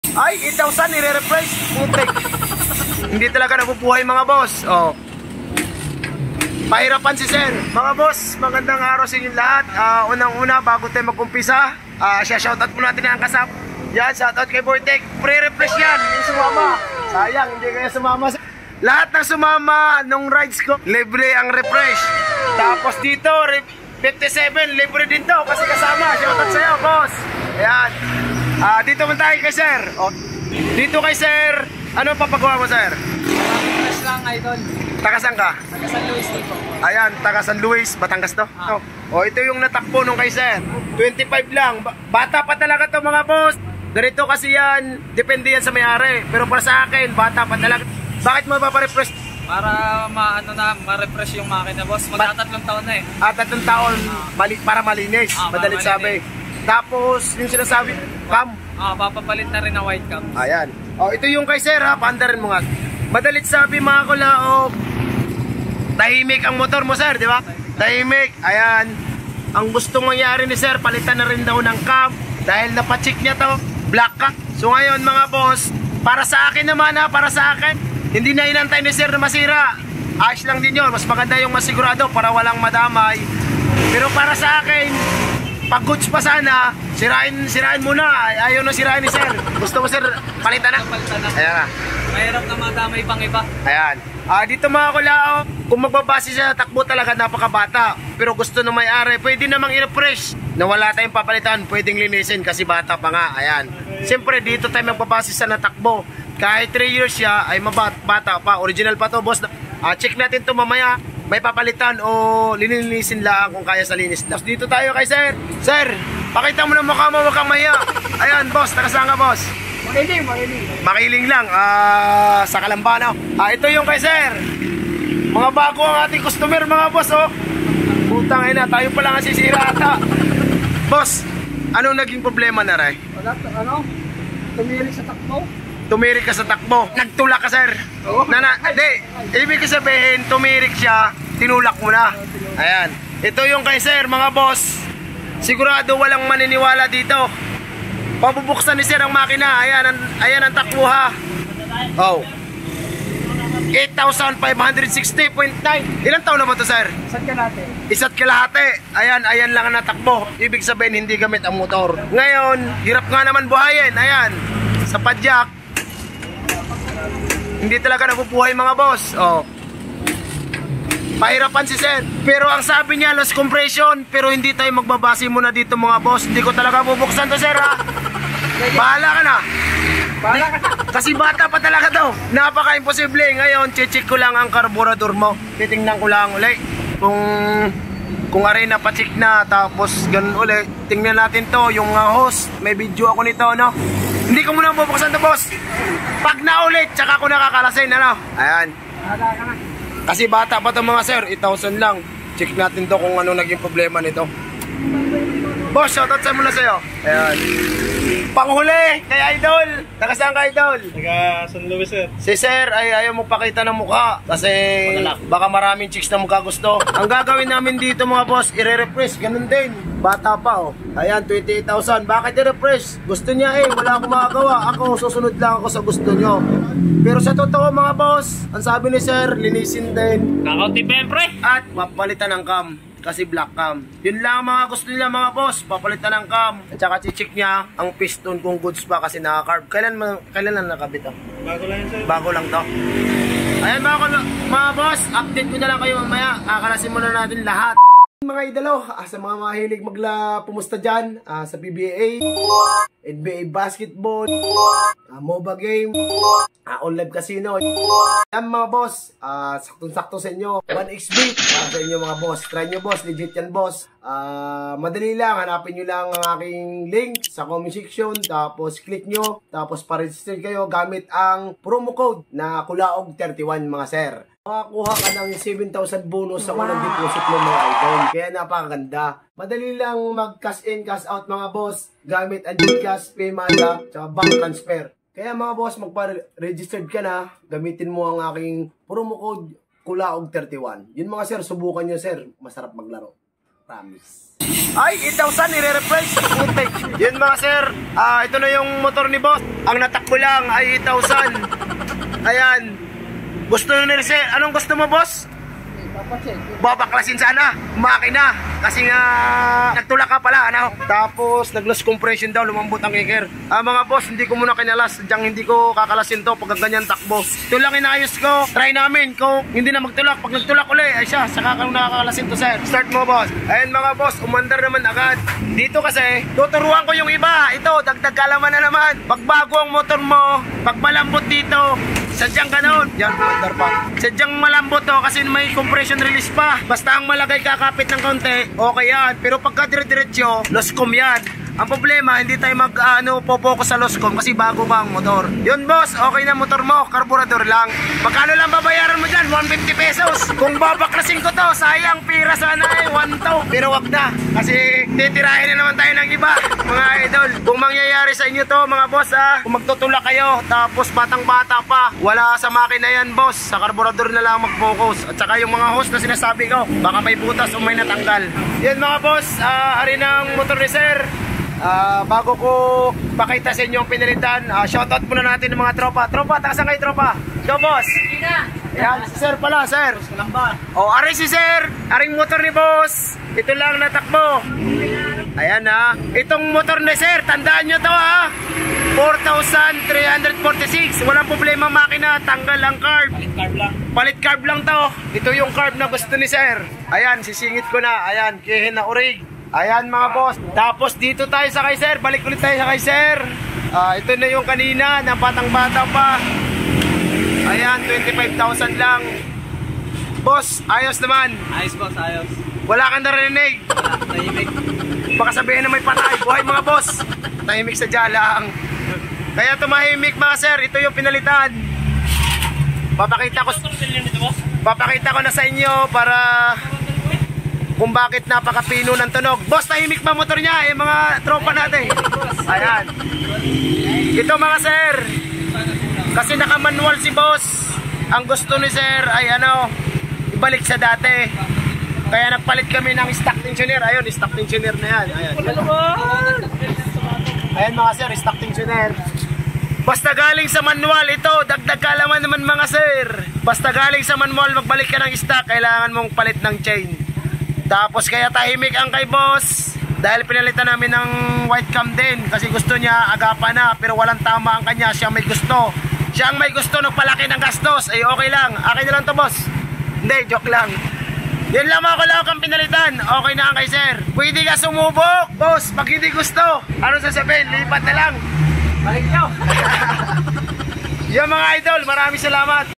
✅TITLE: 8k odo gusto na ni sir pa refresh, panuodin ang fulll video kung bakit ?
Complain ni idol tinirik sya sa daan dina mabuhay at mahirap padyakan,, sabe nya loose comp. daw, 8k ODO palang , gusto na ni sir iparefresh, panuodin ang full video kung bakit,,

✅Social media accounts:
✅tiktok account; Kulaogworks